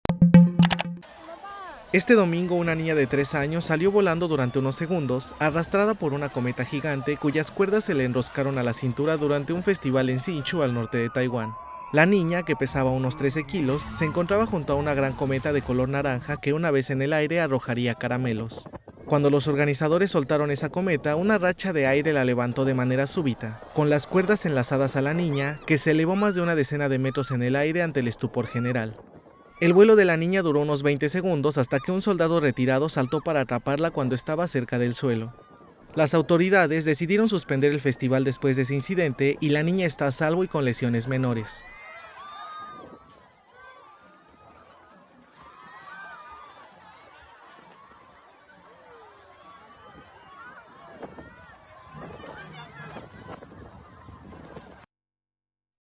Una cometa levanta por el aire a una niña de 3 años en un festival en Taiwán
Una niña de tres años salió accidentalmente volando este domingo durante unos segundos, arrastrada por una cometa gigante cuyas cuerdas se le enroscaron a la cintura durante un festival de cometas en la localidad taiwanesa de Hsinchu, en el norte del país.
¡Visítenos!